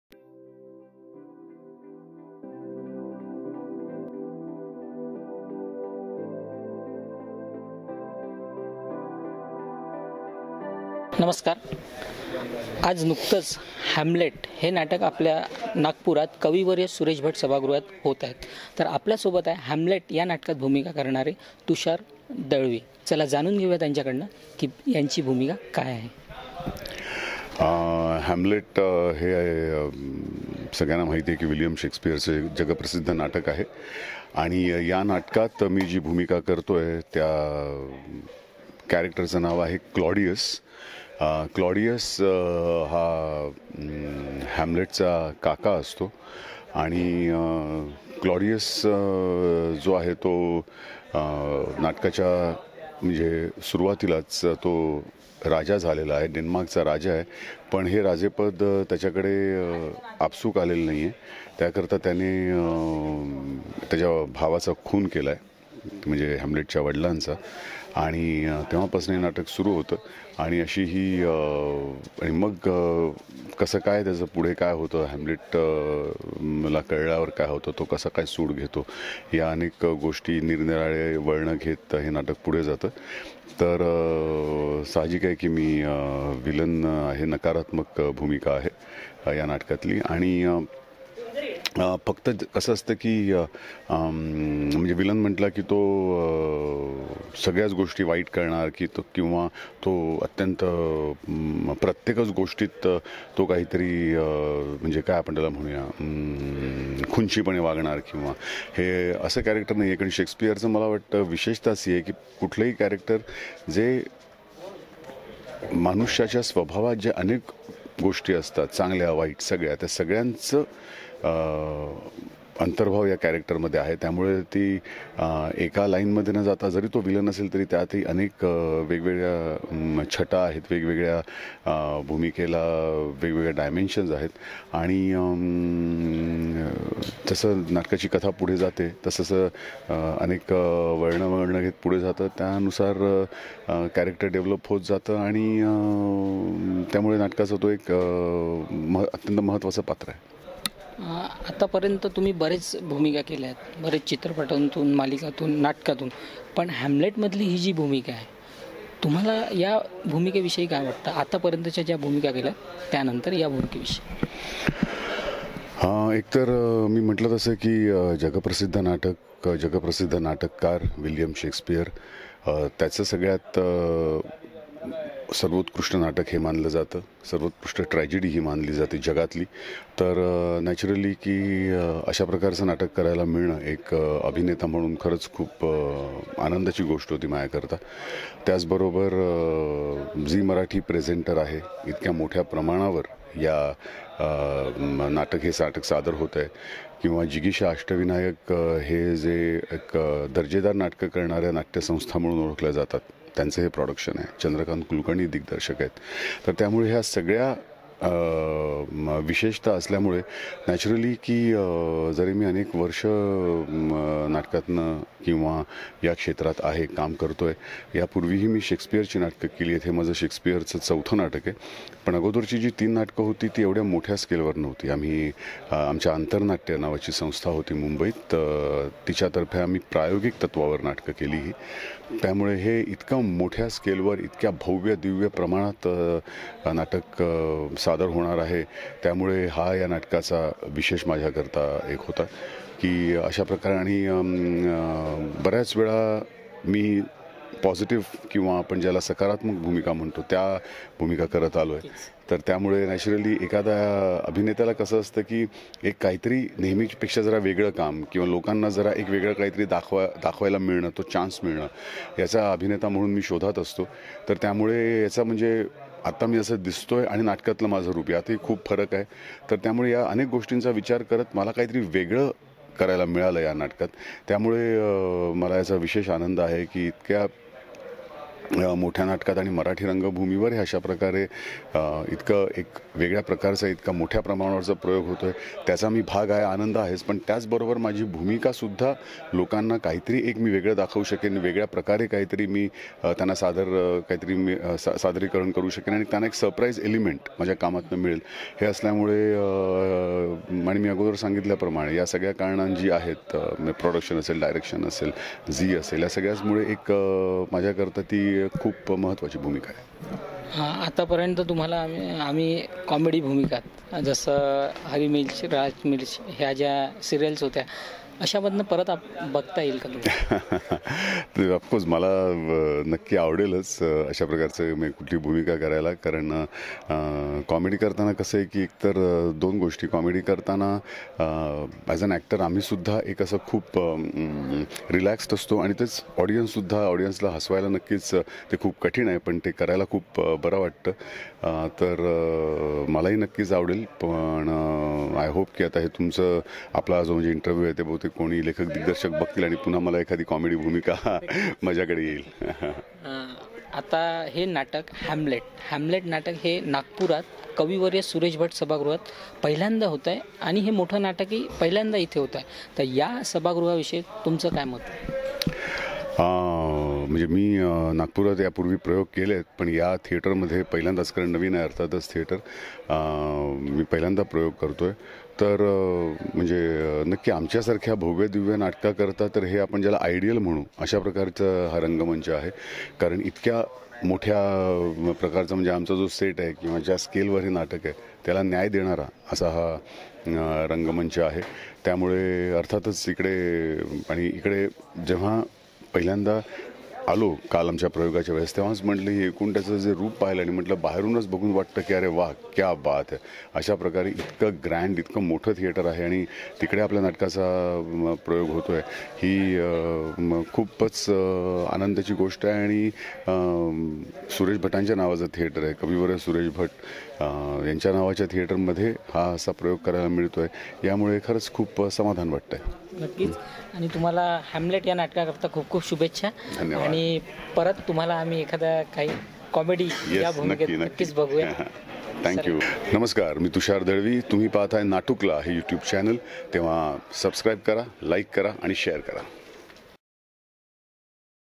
Interview with Tushar Dalviकलावंतांशी सवांद।मराठीWilliam Shakespeare plays Hamlet 1603 |Marathi Play
Interview with TusharDalaviकलावंतांशी सवांद।William Shakespeare Plays Hamlet 1603 | Marathi Play
Hari mirch Lal mirch
DD # National
Holiday , Sarabhai Sarabhai  Aapla Manus Bucket List

Genres: Drama
Rating: na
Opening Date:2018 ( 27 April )
Production House: Jigisha, Ashtavinayak
Producer(s): Dilip Jadhav, Shripad Padmakar
Presenter: Zee Marathi
Original Writer: William Shakespeare
Director: Chadrakant Kulkarni

Credit List

Co-producer(s): na
Director: Chadrakant Kulkarni
Marathi Play Writer: Nana Jog
Dialogues: na
Artist:  Tushar Dalvi, Sunil Tawde, Mugdha Godbole, Manva Naik, Ashish Kulkarni, Bhushan Pradhan, Ranjeet Jog, Omkar Kulkarni, Nitin Bhajan, Sumeet Raghavan
Set Design: Pradeep Mulye
Lights: Pradeep Mulye
Costume: Pradeep Mulye
Swapnil Bansod
Nitin Thakare

Camera &
Editing
Chetan Solanki

Nitesh Shende
Pratik Kulkarni
Sagar Shripal
Rakesh Shende
Aatish Dhoble
Avinash Rangari
Praful Urakude
Payal Dhengre
Nikhil Maske